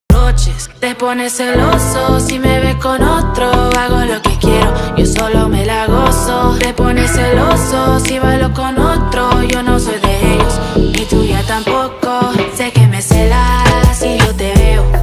Alejandra Sánchez#hospital Gregorio Marañón#2023
Mi pequeña de 5 Años, bailando cuando le dan el alta después de una semana internada en el hospital.